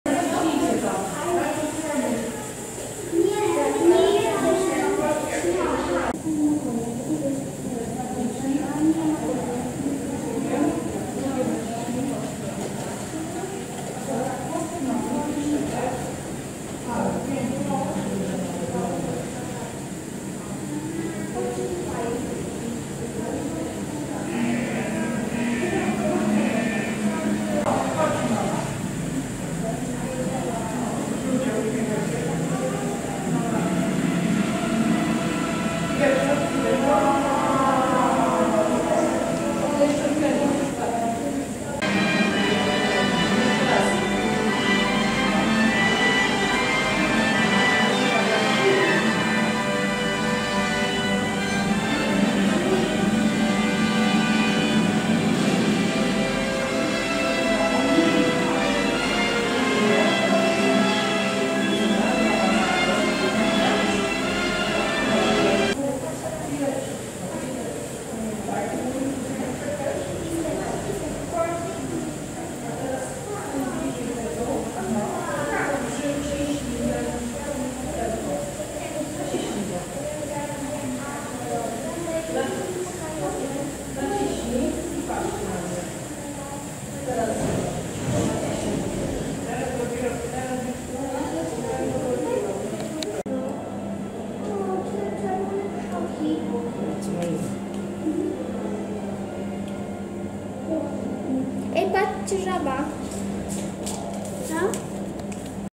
Powrót do dzieciństwa z klockami Lego
Dla wielu osób to może być powrót do czasów dzieciństwa. Filmik z Karpacza.
Zapraszamy do subskrypcji i dania łapki w górę !